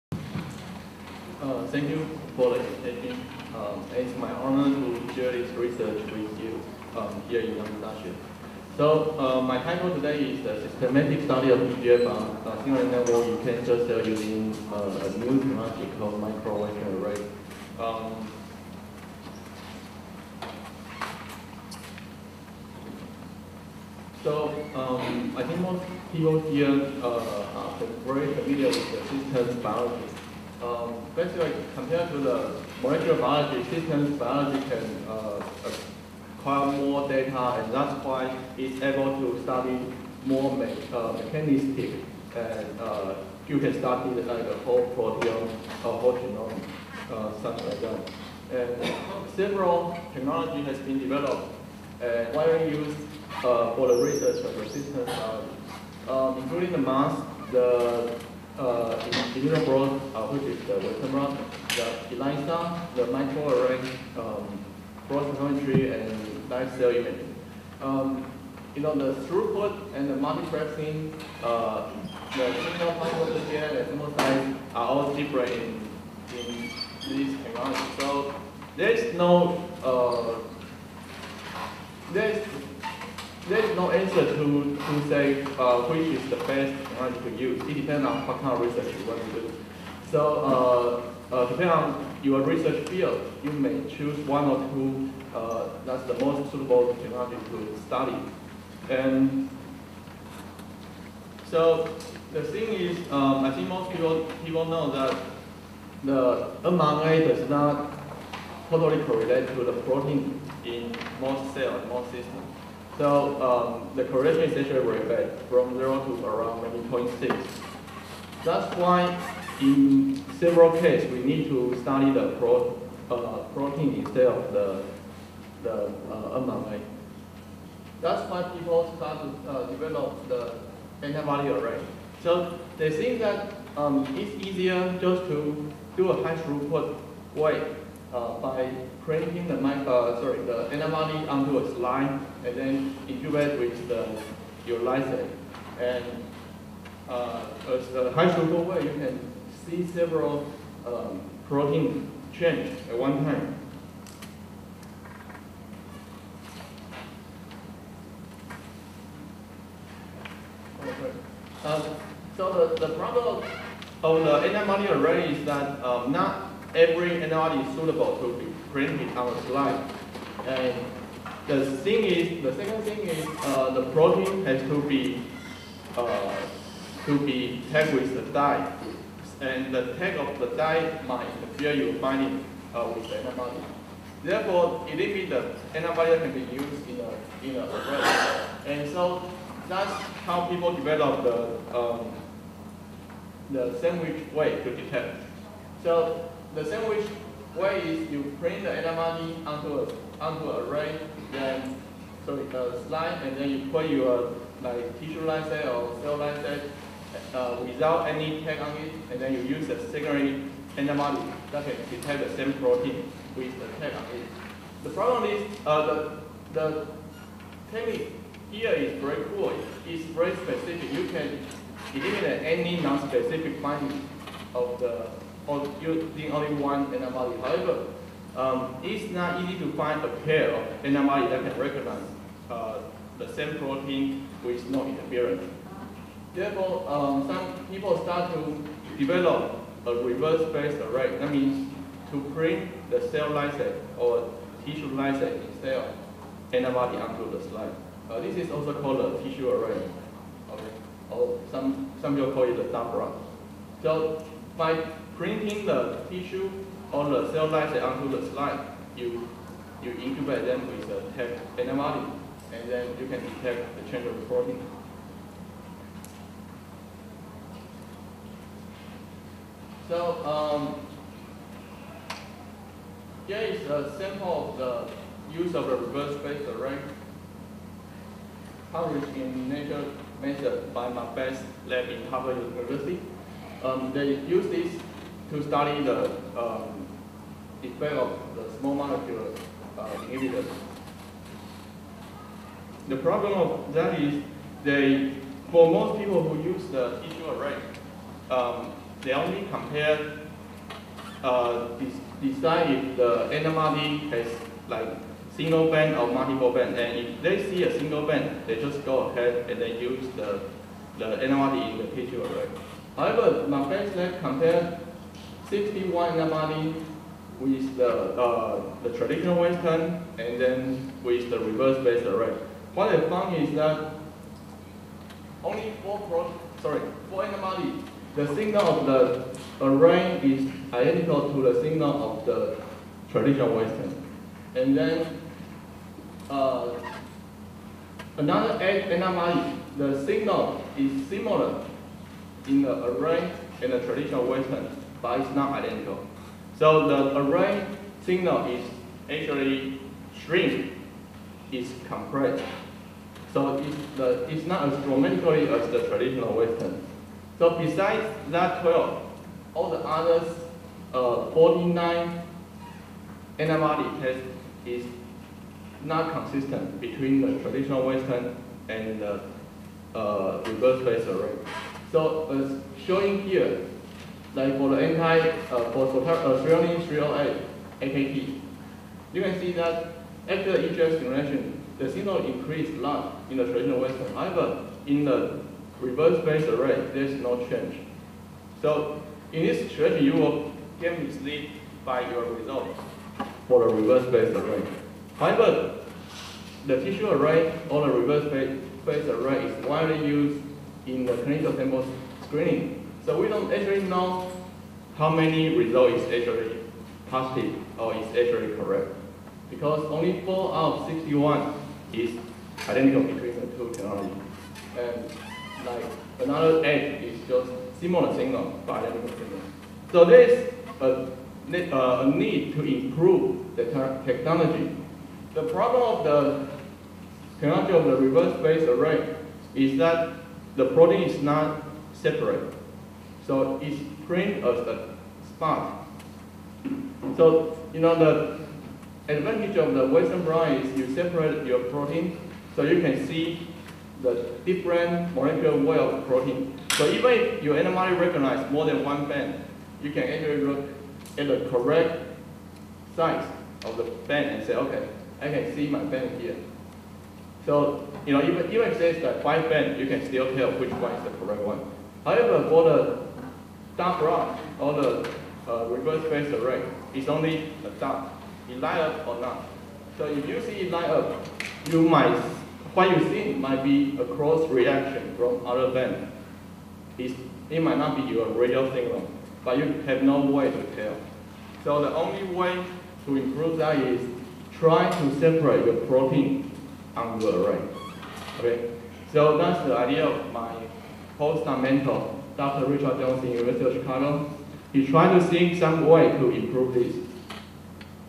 Microwestern Arrays - Lecture Series 1
) - Lecture series 1 compares different array technologies and reveals their limitations and current challenges which brings out the importance of microwestern arrays. Conference: 8th Bioinformatics and Systems Biology in Taiwan (BIT) Systems analysis of signaling pathways in human cancer cells with high-throughput western blotting assay-the microwestern arrays (Dr. Chih-Pin Chuu). Dr. Chuu developed the Microwestern arrays, a high throughput western blotting assay, which enables quantitative, sensitive and high throughput assessment of protein abundance and modifications after electrophoretic separation of microarrayed cell lysates. More videos at Abnova